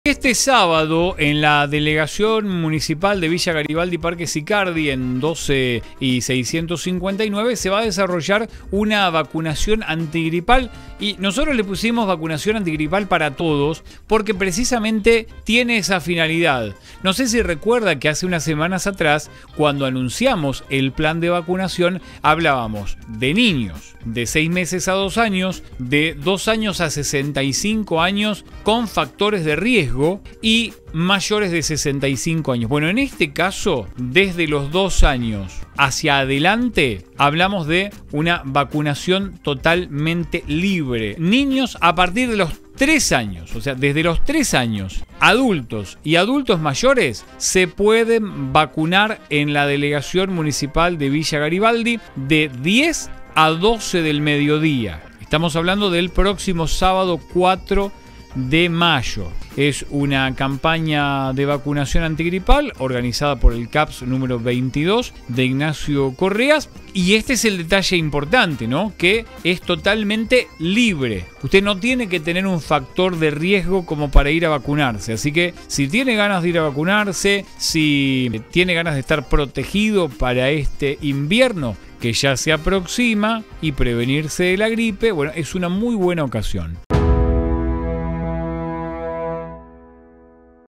Vacunacion en la delegación de Villa Garibaldi
📢 ¡Atención comunidad de Villa Garibaldi, Parque Sicardi! Este sábado 4 de Mayo, se llevará a cabo una campaña de vacunación antigripal para todas las edades en la Delegación Municipal. Organizada por el CAPS Número 22 de Ignacio Correas, esta iniciativa ofrece la oportunidad de protegerse del virus de la gripe sin restricciones. ¡Vacunate para un invierno más saludable!